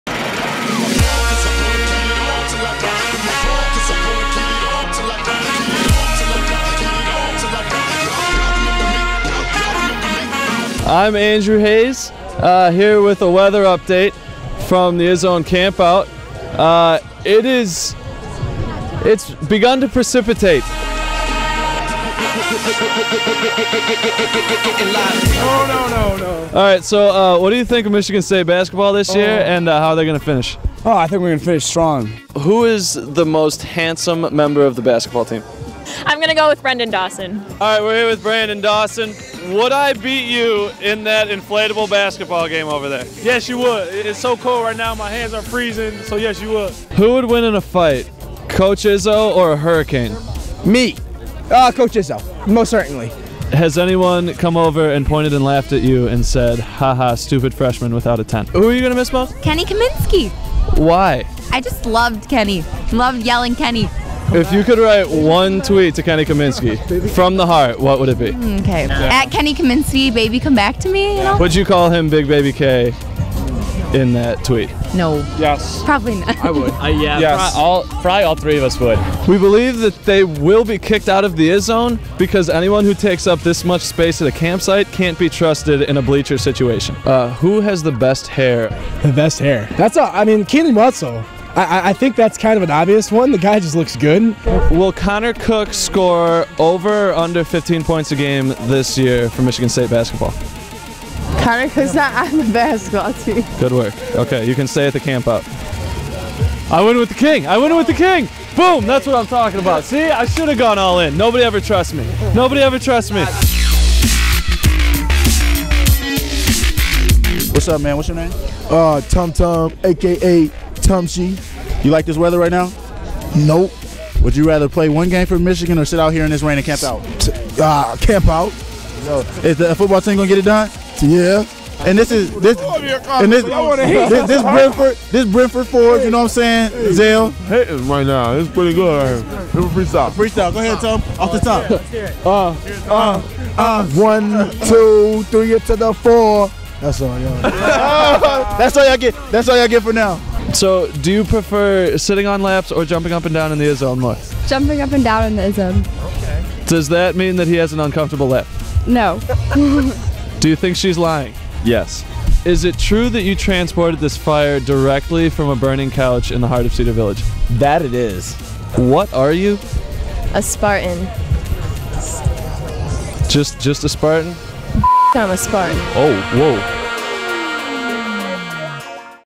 Izzone Campout 2014
Denzel Valentine, Tum Tum Nairn, Javon Bess and other Michigan State basketball players have some fun at the 2014 Izzone Campout!

Through rain or shine, students do whatever it takes to support MSU basketball and get a spot in one of the loudest student sections in the nation. Host Andrew Hayes goes on adventures into the tents of numerous fans on October 17, 2014.

Edit: Scott Wasserman and Alex Scharg
Camera: Scott Wasserman
Host: Andrew Hayes